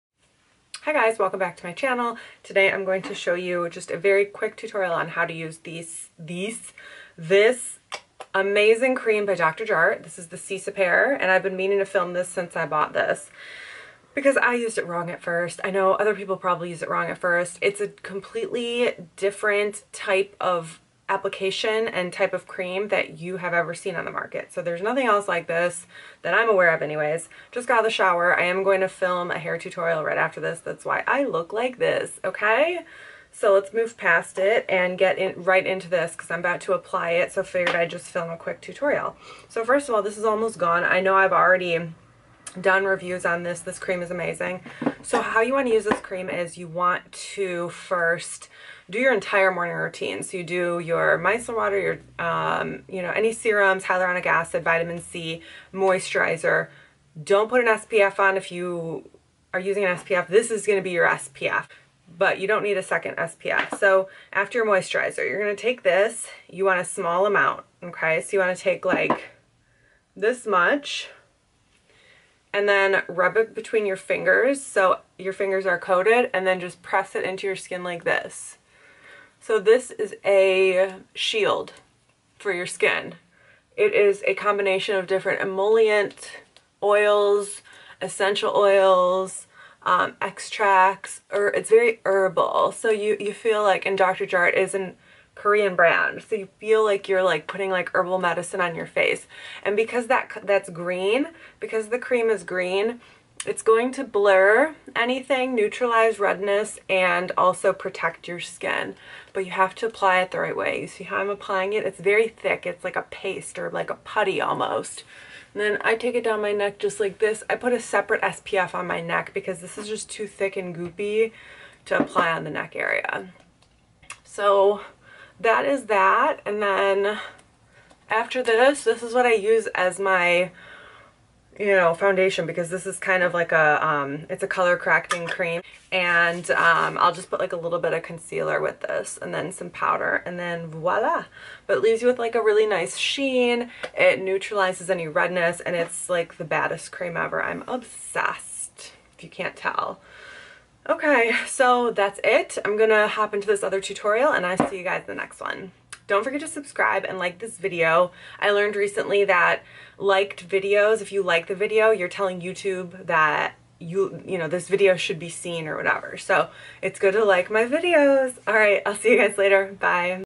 BEST CREAM!! DR JART Cicapair Tiger Grass Color Correcting Treatment | HOW TO USE
EDIT: there are a lot of questions about whether this amount of product is enough to provide proper SPF protection. For additional protection I would recommend wearing an SPF under this cream but not adding more of this cream. However I’m NOT A DOCTOR this is just my personal opinion!! Xoxo

Dr Jart Cicapair Color Correcting Cream is amazing I love it. But you have to apply it properly otherwise it looks like shit on the skin. This goes on in your MORNING routine after your moisturizer!!

Social Medias
💜Podcast: Jenny Talks on itunes

Dermaroller (for acne scars) I recommend: